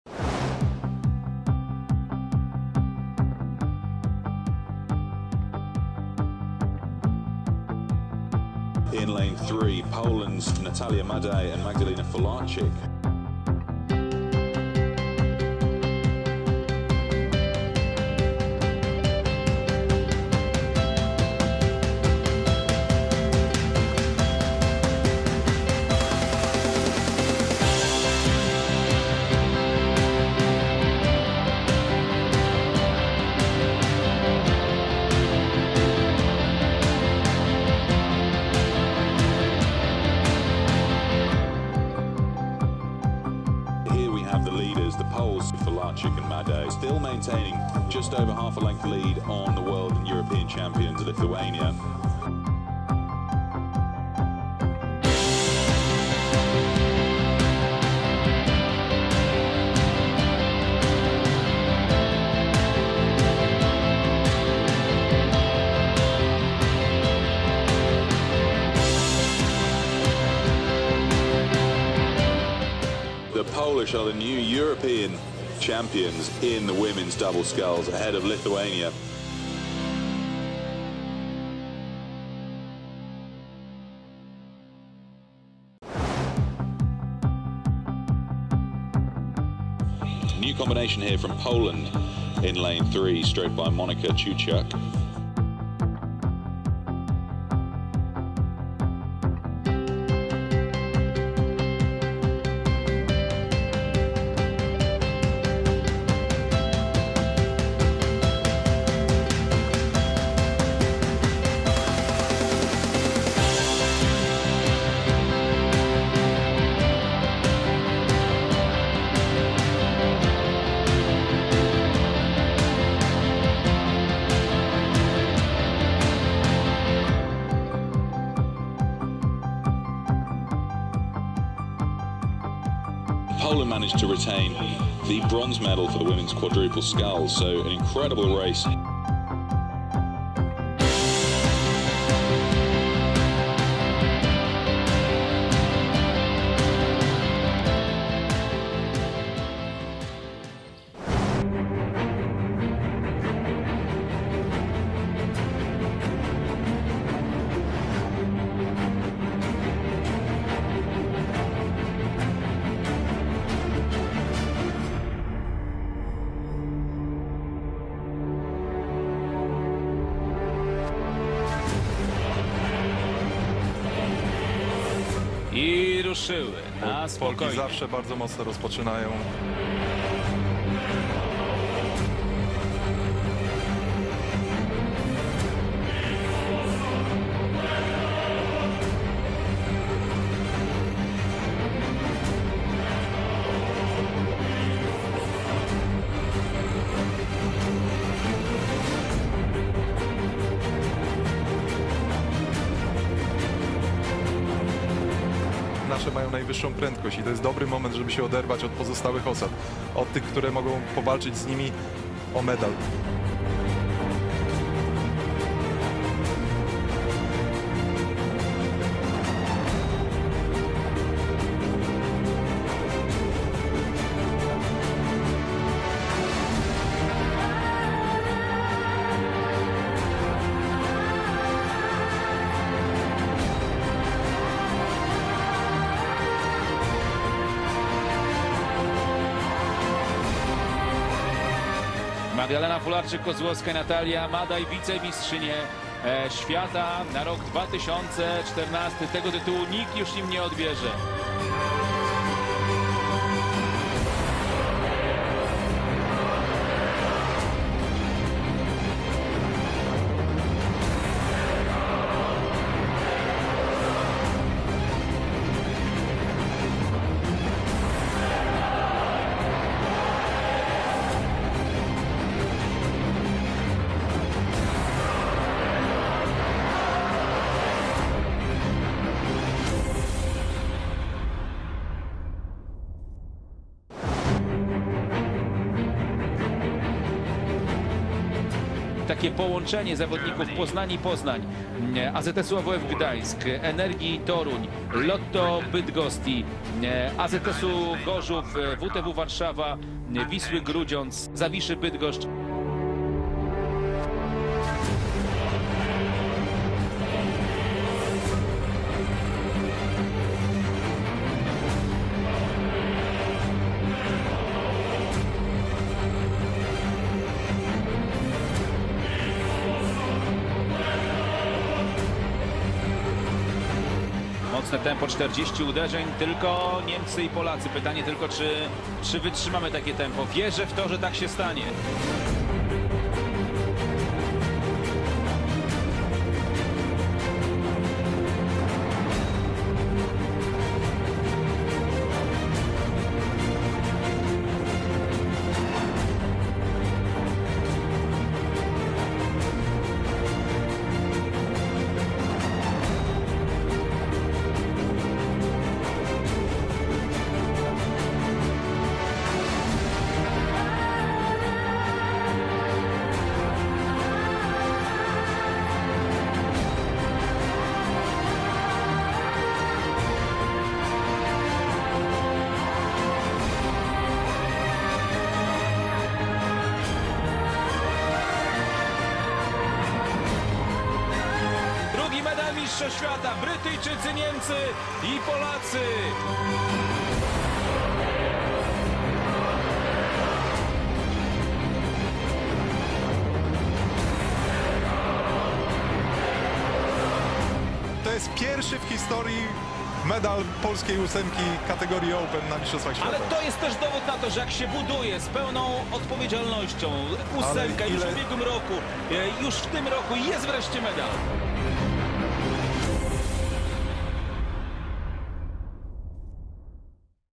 POLISH ROWING MEDALS IN EUROPEAN AND WORLD CHAMPIONSHIPS 2014
W2x - GOLD MEDAL, BELGRADE 2014
W4x - BRONZE MEDAL, BELGRADE 2014
W2x - SILVER MEDAL, AMSTERDAM 2014
M8+ - BRONZE MEDAL, AMSTERDAM 2014